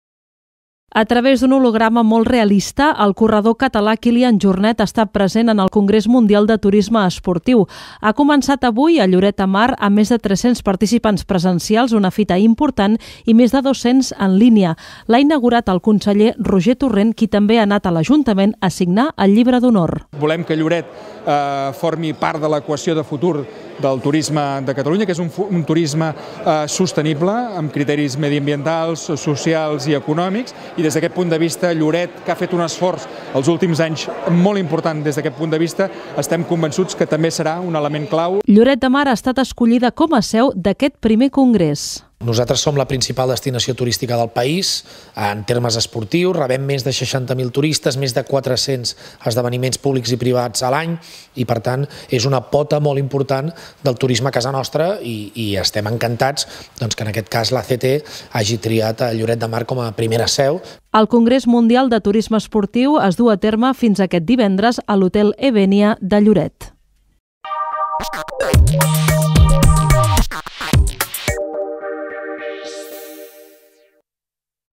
El present i futur del turisme esportiu, a debat a Lloret de Mar
La destinació acull el primer Congrés Mundial de Turisme Esportiu, amb Kilian Jornet a través d'un holograma molt realista.